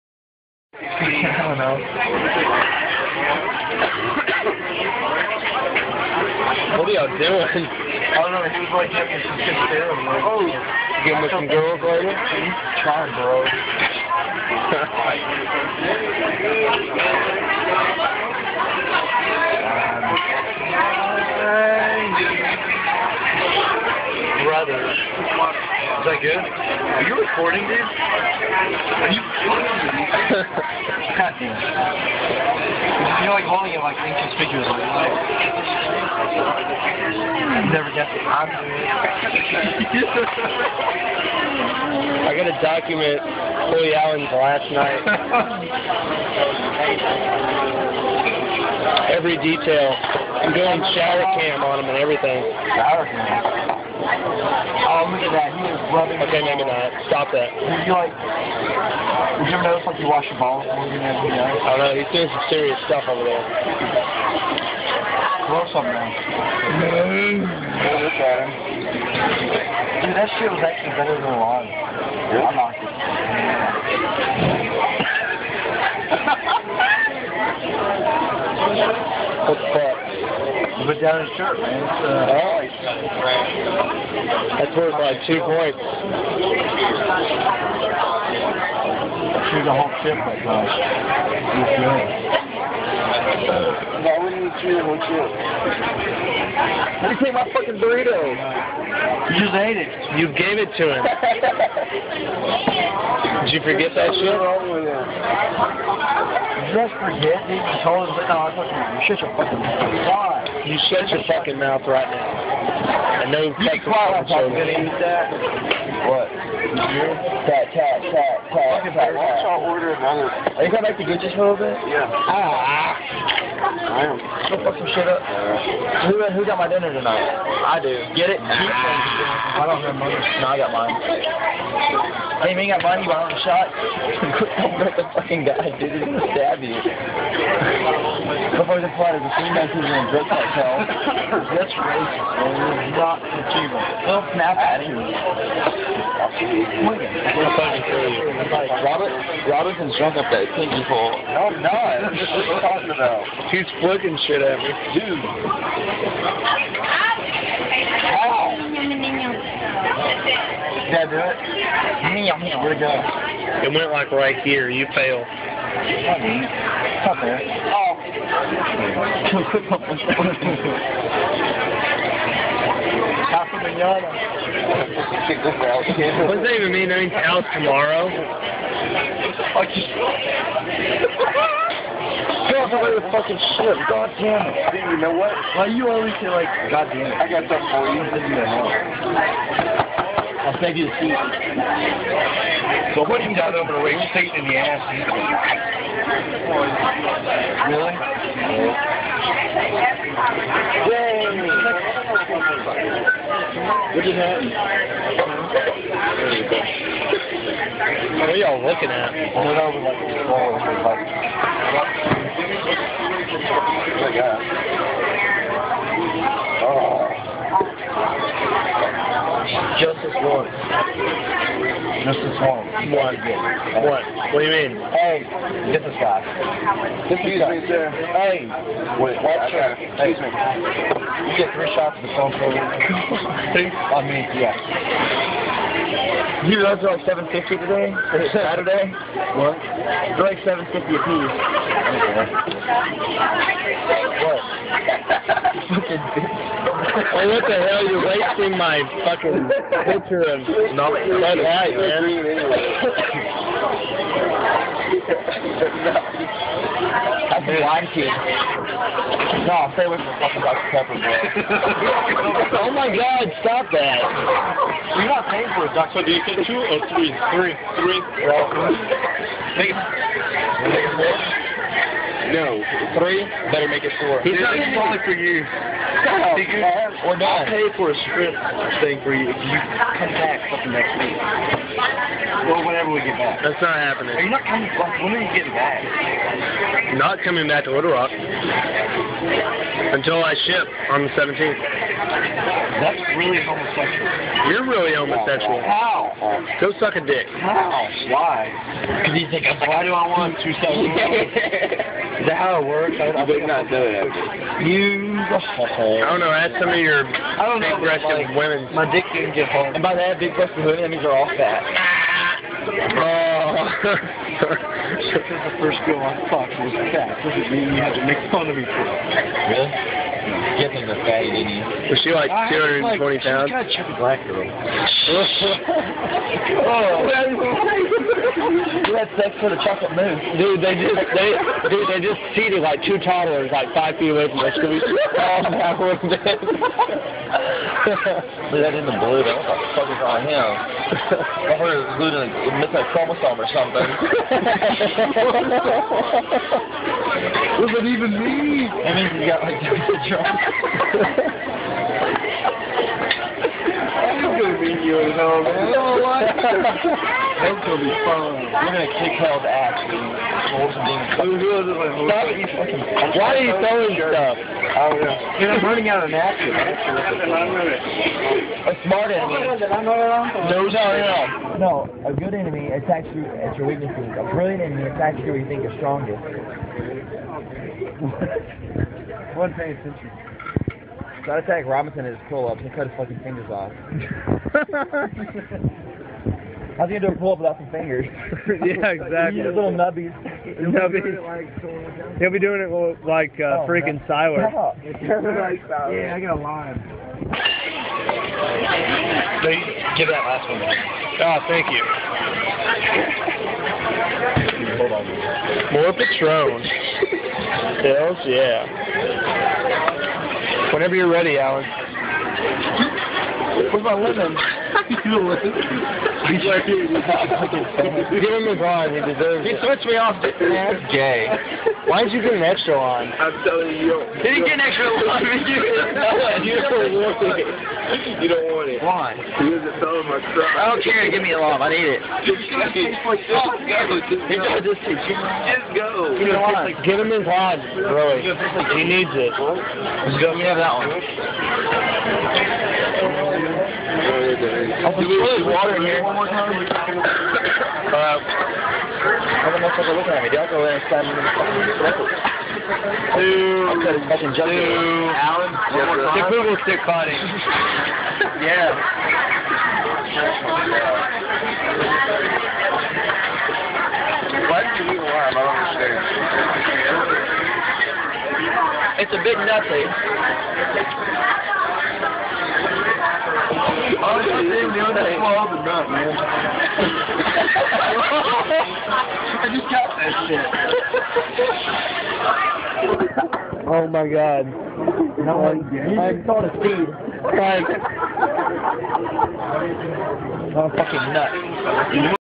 Allen's Last Night
Eating at El Porton, having a little tequila :D